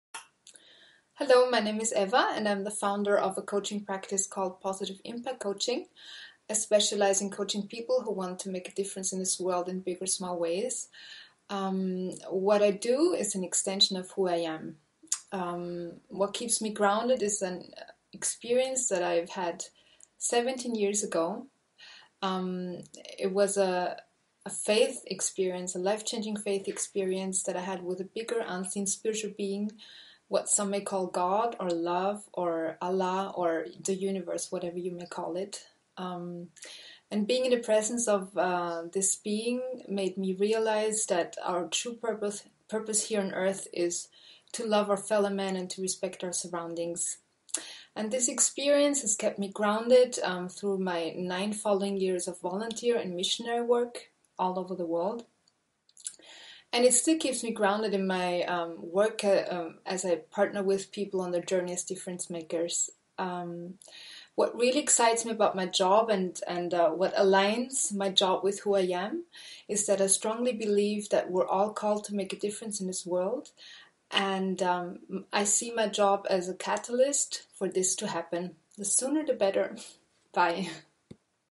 24More - We are all called to make a difference
Eva Berger, Positive Impact.

This video is part of the 24more campaign, promoted by Amani Institute and iLEAP to celebrate the gift of 24 more hours in Leap Year 2016 in service of a better alignment of our purpose and our career. Do you want to join us?
and spread the message 24more.